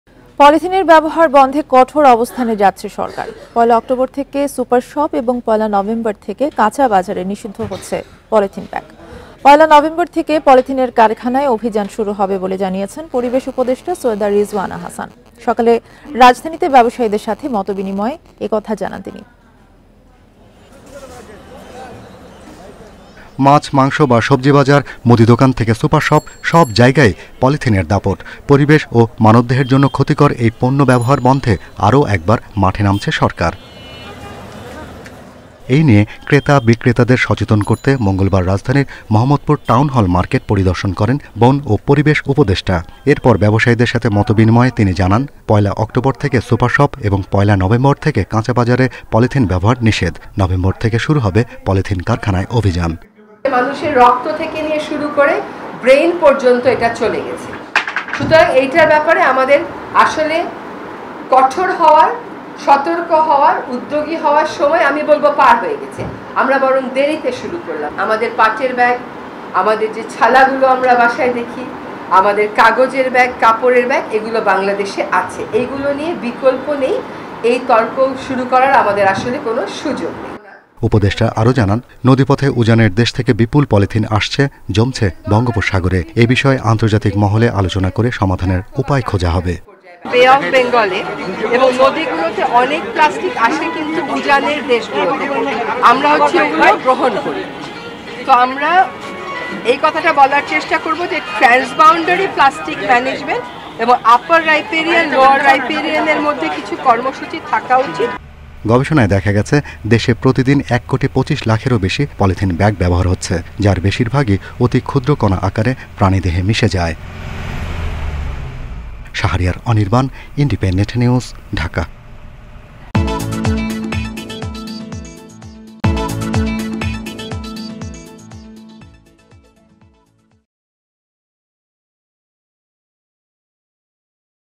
নভেম্বর থেকে কাঁচাবাজারেও পলিথিন নিষিদ্ধ, অভিযান হবে কারখানাতেও | Polythene Ban | Independent TV
পলিথিনের ব্যবহার বন্ধে কঠোর অবস্থানে যাচ্ছে সরকার। পয়লা অক্টোবর থেকে সুপার শপ এবং পয়লা নভেম্বর থেকে কাঁচাবাজারে নিষিদ্ধ হচ্ছে পলিথিন ব্যাগ। পয়লা নভেম্বর থেকে পলিথিনের কারখানায় অভিযান শুরু হবে বলে জানিয়েছেন পরিবেশ উপদেষ্টা সৈয়দা রিজওয়ানা হাসান। সকালে রাজধানীতে ব্যবসায়ীদের সাথে মতবিনিময়ে একথা জানান তিনি।

The government is taking a strict stand to stop the use of polythene. Polythene bags are banned in super shops from October 1 and raw markets from November 1. Environment advisor Syeda Rizwana Hasan said that the campaign will start in the polythene factory from November 1. He said this in an exchange of views with businessmen in the capital in the morning.